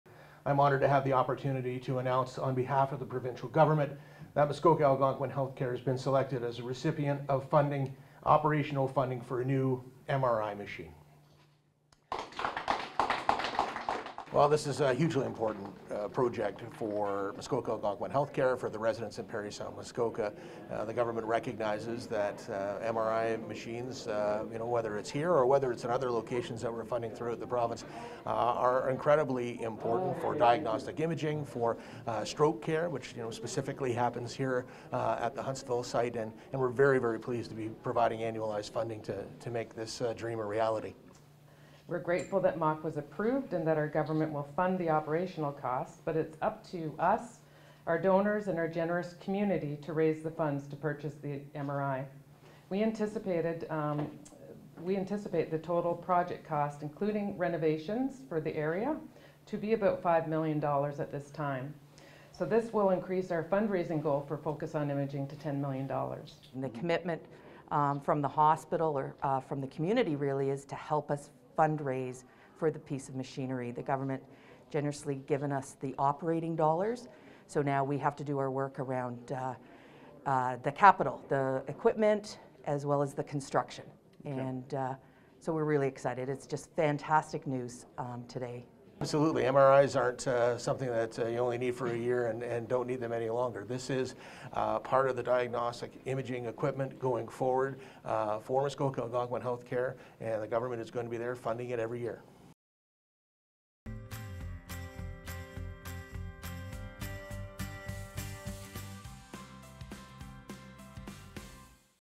An MRI machine is in Muskoka's healthcare future
The Ontario Government is investing in operational funding for an MRI machine for Muskoka. But, it will be up to the community to cover the cost of the machine.

Muskoka Algonquin Healthcare Graydon The MPP Huntsville Hospital Foundation Town of Huntsville, Muskoka, Ontario